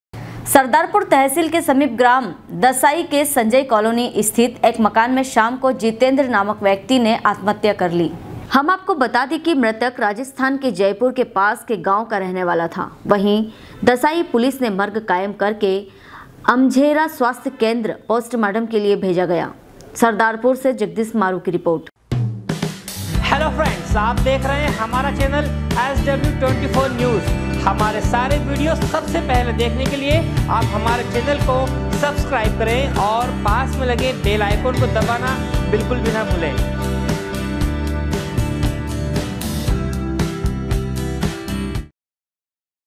सरदारपुर ग्राम दसाई मे संजय कॉलोनी स्तिथ एक मकान मे शाम को जीतेंद्र नामक व्यक्ति ने आत्महत्या
सरदारपुर तहसील के समीप ग्राम दसाई मे संजय कॉलोनी स्तिथ एक मकान मे शाम को जीतेंद्र नामक व्यक्ति ने आत्महत्या कर ली